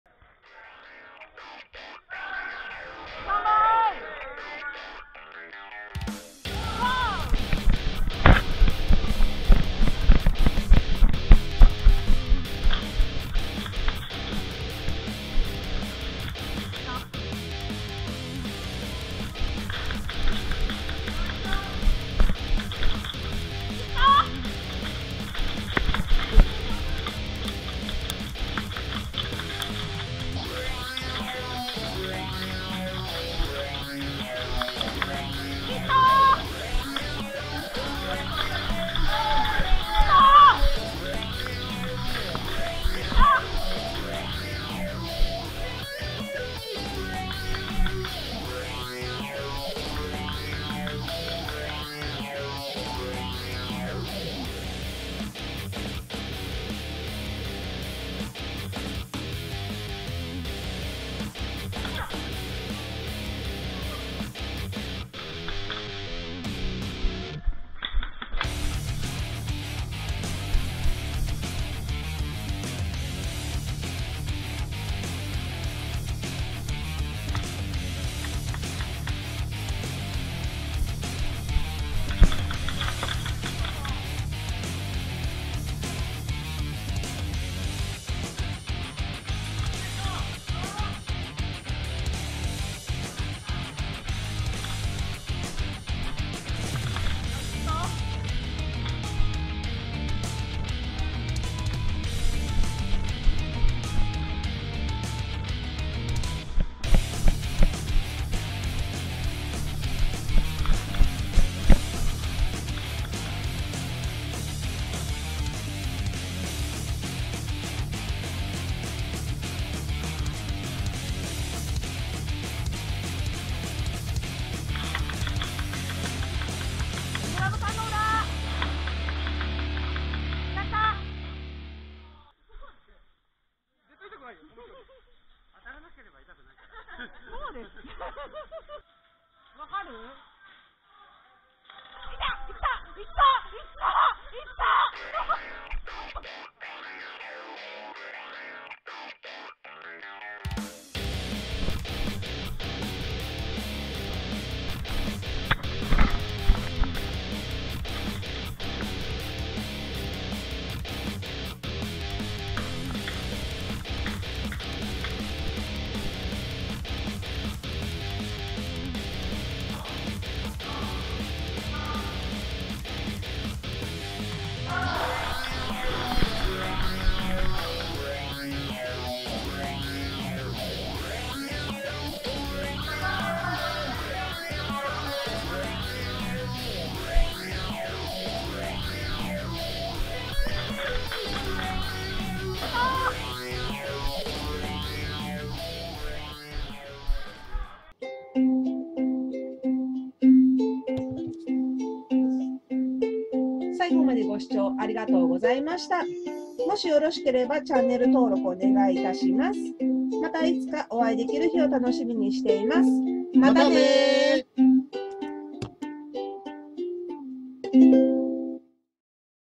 [HQバンカー]H31.3.31 初菜のサバゲー日記＃12[サバゲー]
今回もＨＱバンカーでサバゲーしてきました
まとめ動画ですが見ていってください

音源に魔王魂様使用しています